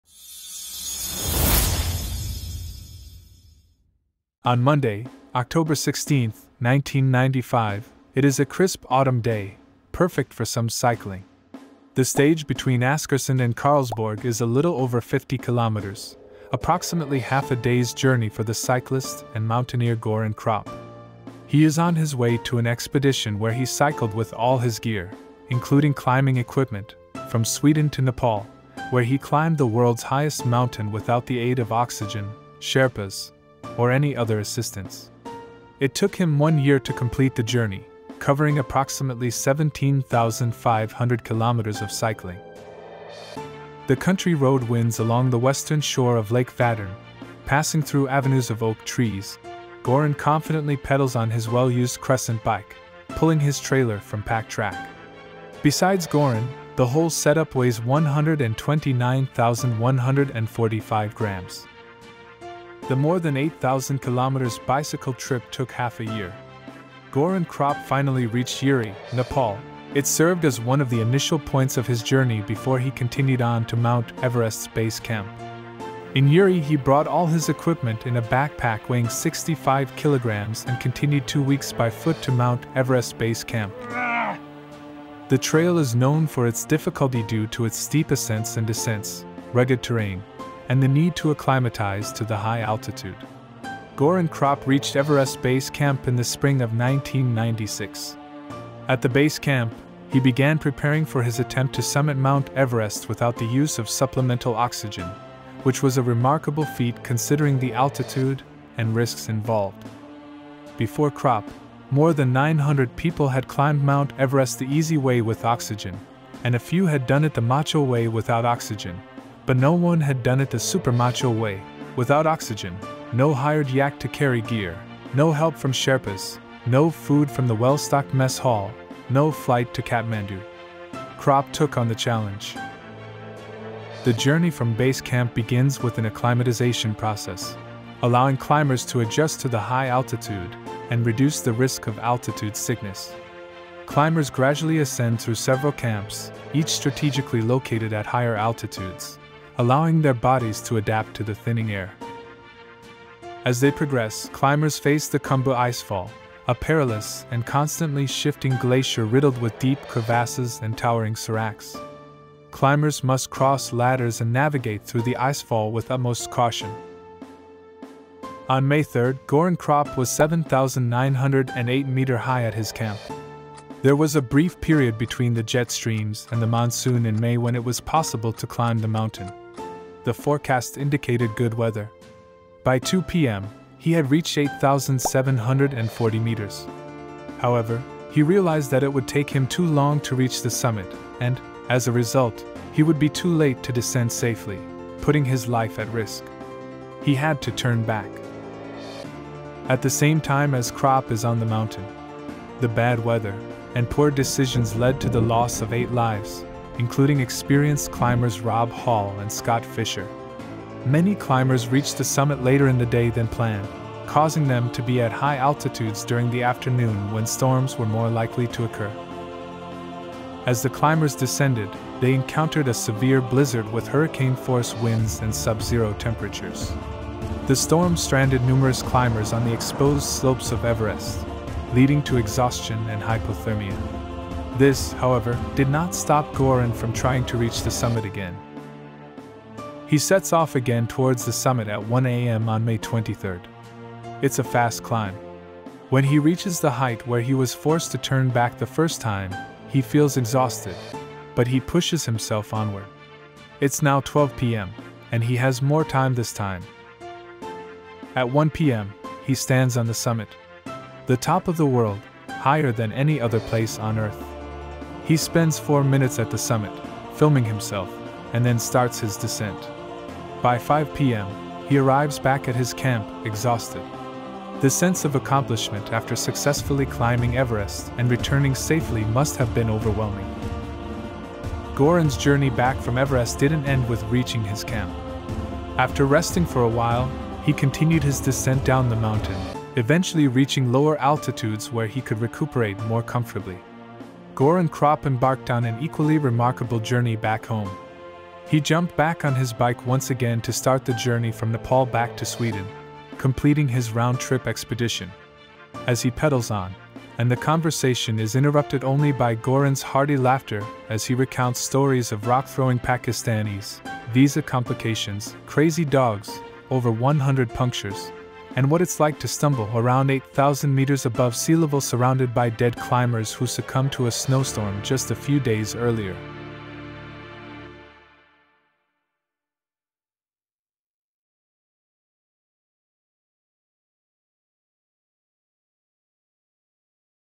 Everest- The fascinating summit climber 😧🧗‍♂️(Göran Kropp)
In this video, we'll explore the incredible adventure of Göran Kropp as he embarks on a groundbreaking expedition to Mount Everest - by bike! Join us as we delve into the awe-inspiring journey of this remarkable individual who dared to conquer the world's highest peak in the most extraordinary way possible.

We'll take you through Göran Kropp's awe-inspiring story of determination, courage, and sheer willpower as he cycles thousands of kilometers to reach the foot of Everest. Witness the breathtaking landscapes, the challenges he faced, and the triumphs he achieved, all in the pursuit of his dream to stand atop the world.

From the dusty roads to the icy heights, this expedition epitomizes the true spirit of adventure. If you're fascinated by Everest, inspired by remarkable human feats, and eager to witness the thrill of conquering the impossible, then you've come to the right place.